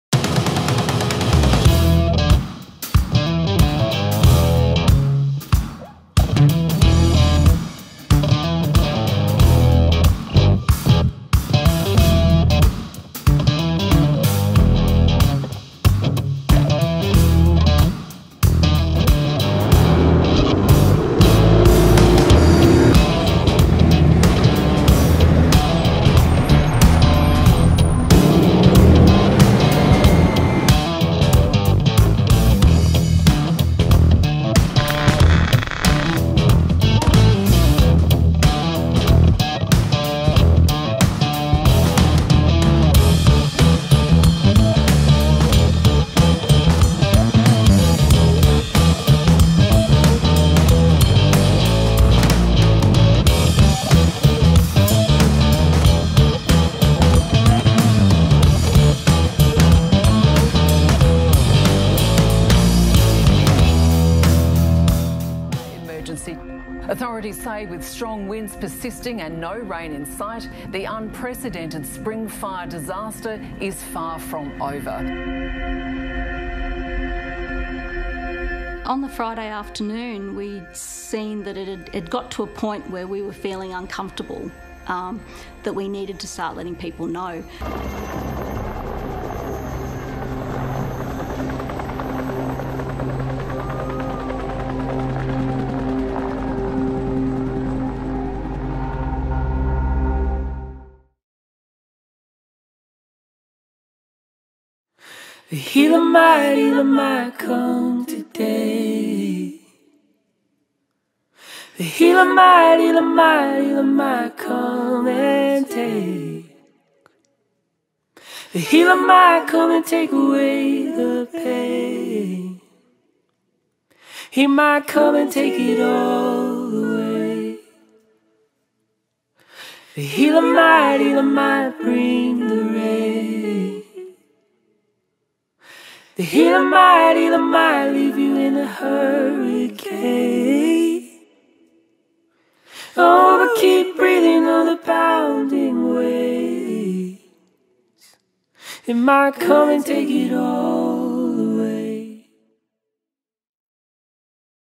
Shipping Container homes - Habitable Living Spaces
Time Lapse Container home build - Australia NSW.
- 6 Shipping Containers come together to make one GREAT home.